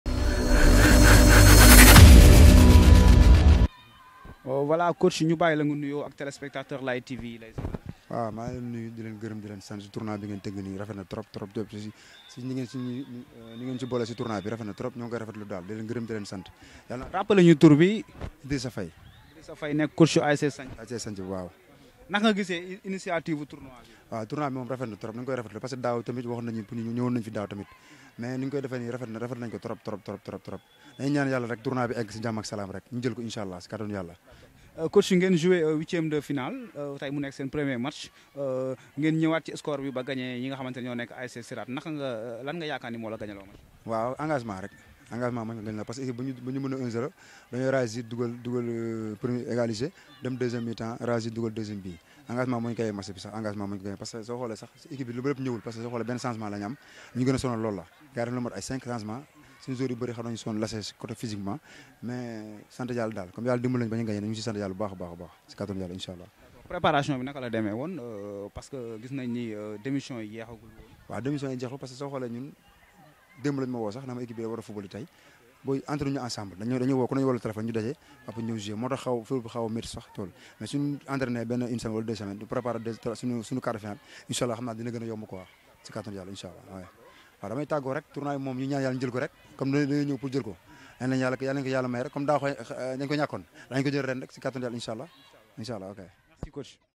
Idrissa Faye coach Asc Sanc Bi très content félicite 2 AFP et ses joueurs sur la victoire
Inter saison sur light tv